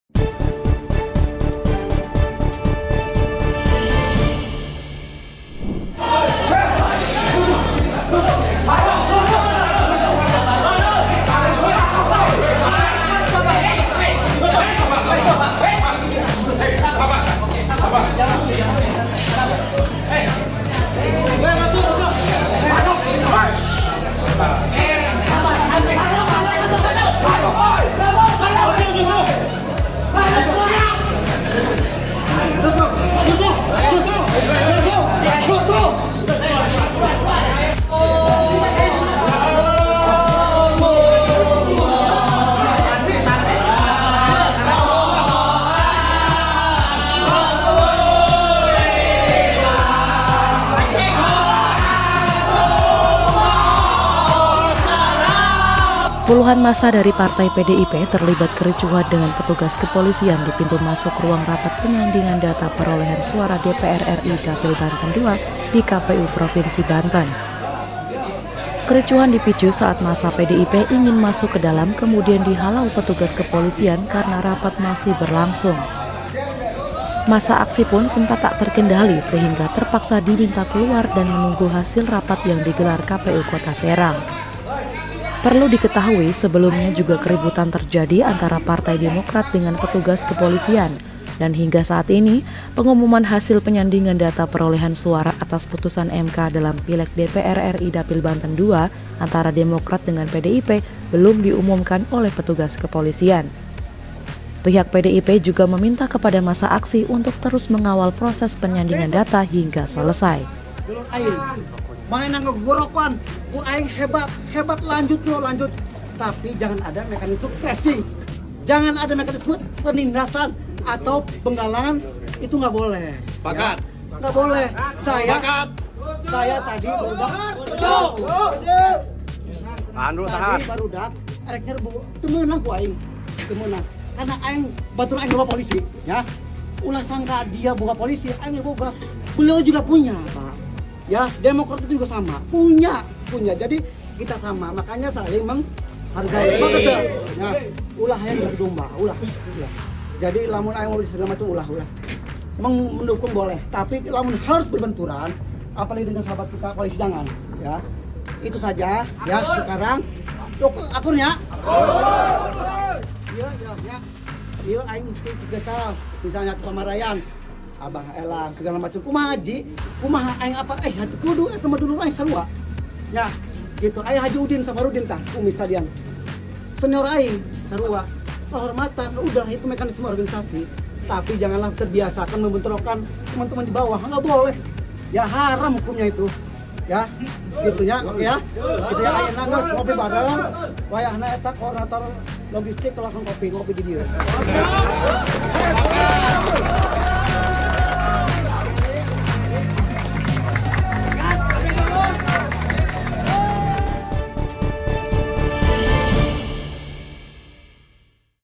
Memaksa Masuk Ruang Rapat Penyandingan Data, Massa PDIP Ricuh | OneNews Update
OneNews Update, - Memaksa Masuk Ruang Rapat Penyandingan Data, Massa PDIP Ricuh | OneNews Update

Puluhan massa PDIP ricuh dengan pihak kepolisian saat mencoba masuk dalam rapat penyandingan data perolehan suara DPR RI Dapil Banten Dua di KPU Provinsi Banten
(REG01) (RTR01)

AHM01
NU01